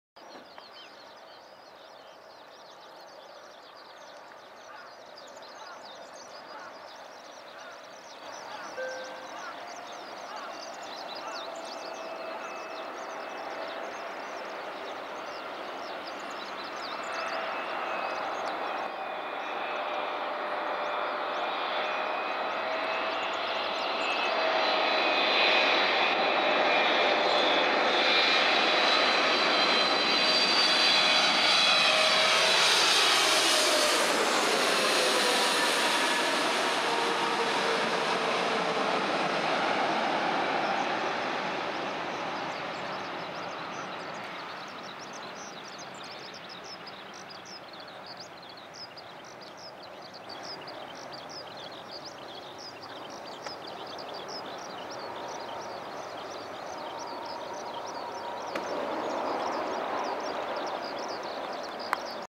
Vapour !!! British Airways B777 landing @Narita Airport
Everio Gz-E565使用。2013年6月15日。
成田空港Brwy16Lエンドにて。